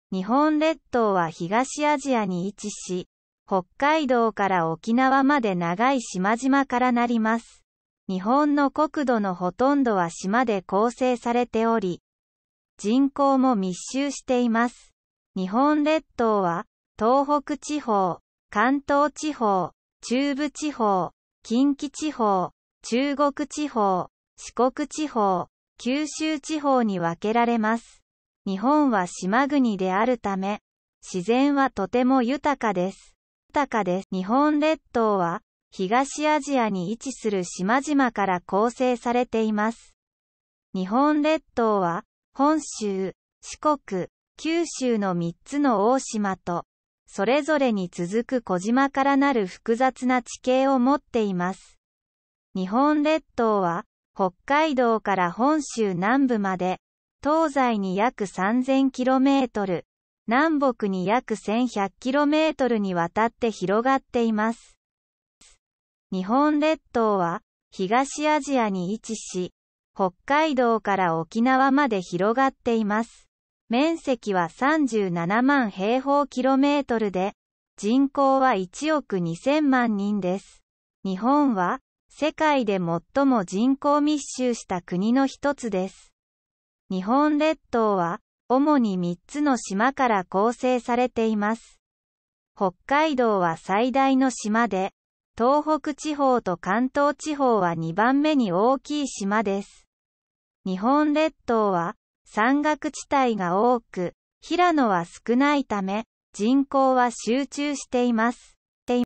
Learn JAPANESE |The Japanese Archipelago and its Geography(translation in description)
The Japanese Archipelago and its Geography
BEGINNER
JAPANESE
日本列島は東アジアに位置し、北海道から沖縄まで長い島々から成ります。日本の国土のほとんどは島で構成されており、人口も密集しています。日本列島は、東北地方、関東地方、中部地方、近畿地方、中国地方、四国地方、九州地方に分けられます。日本は島国であるため、自然はとても豊かです。
ENGLISH
The Japanese archipelago is located in East Asia, and consists of a chain of islands stretching from Hokkaido to Okinawa. Most of Japan's territory is made up of islands, and the population is also dense. The Japanese archipelago is divided into the Tohoku region, the Kanto region, the Chubu region, the Kinki region, the Chugoku region, the Shikoku region, and the Kyushu region. Because Japan is an island country, nature is very abundant.
INTERMEDIATE
JAPANESE
日本列島は、東アジアに位置する島々から構成されています。日本列島は、本州、四国、九州の3つの大島と、それぞれに続く小島からなる複雑な地形を持っています。日本列島は、北海道から本州南部まで、東西に約3000km、南北に約1100kmにわたって広がっています。
ENGLISH
The Japanese Archipelago is made up of islands located in East Asia. The Japanese Archipelago has a complex topography consisting of three large islands of Honshu, Shikoku, and Kyushu, and small islands following each of them. The Japanese Archipelago extends approximately 3000 km from Hokkaido to the southern part of Honshu and1100 km from north to south.
ADVANCED
JAPANESE
日本列島は、東アジアに位置し、北海道から沖縄まで広がっています。面積は37万平方キロメートルで、人口は1億2000万人です。日本は、世界で最も人口密集した国の1つです。日本列島は、主に3つの島から構成されています。北海道は最大の島で、東北地方と関東地方は2番目に大きい島です。日本列島は、山岳地帯が多く、平野は少ないため、人口は集中しています。
ENGLISH
The Japanese archipelago is located in East Asia and spans from Hokkaido to Okinawa. It has an area of 370,000 square kilometers and a population of 120 million people. Japan is one of the most densely populated countries in the world. The Japanese archipelago is composed mainly of three islands. Hokkaido is the largest island, and the Tohoku and Kanto regions are the second largest islands. The Japanese archipelago has many mountainous areas and few plains, so the population is concentrated.